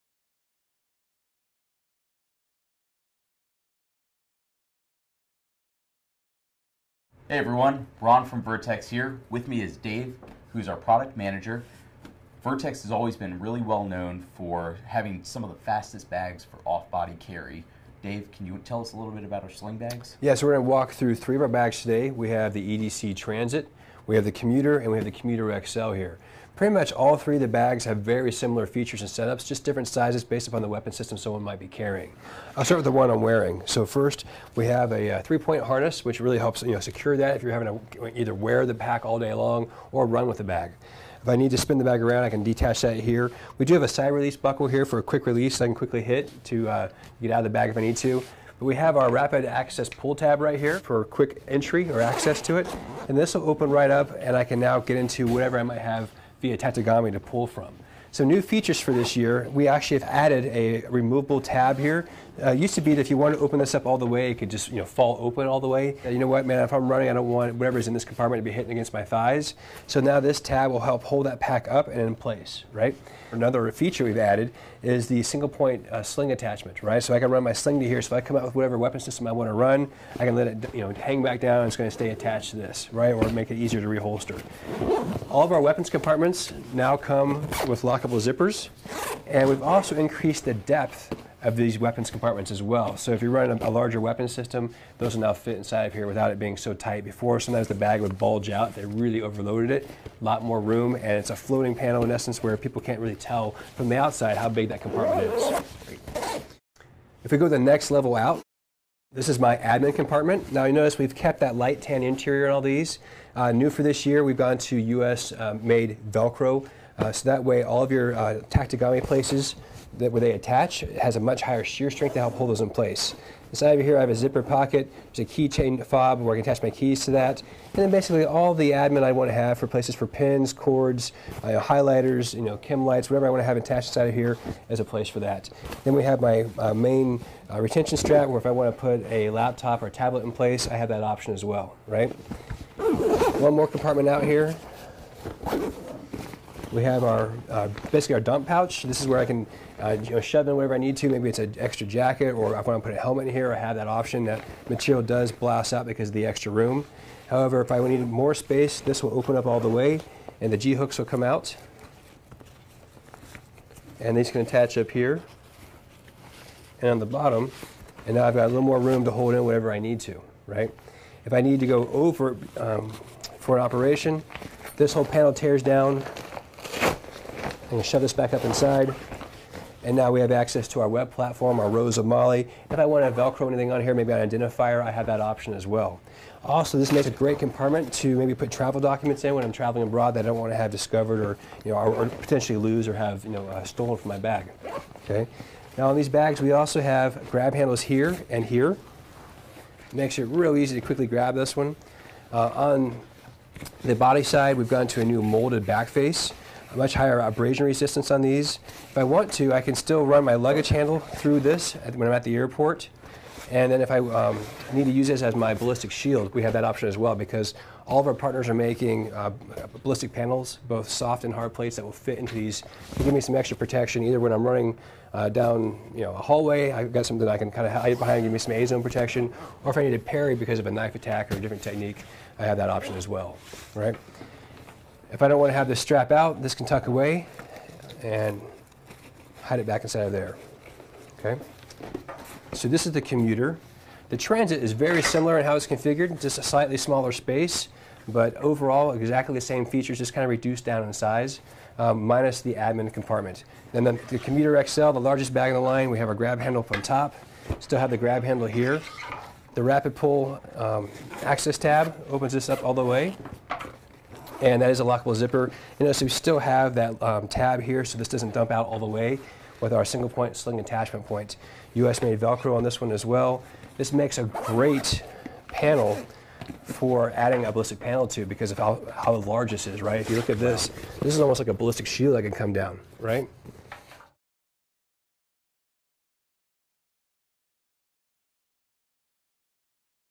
Vertx Sling Bags Overview
Der Vertx Transit Sling 2.0 ist eine geräumige und vielseitige Schultertragetasche mit unauffälligem Design, welche alle CCW- und EDC-Anforderungen erfüllt.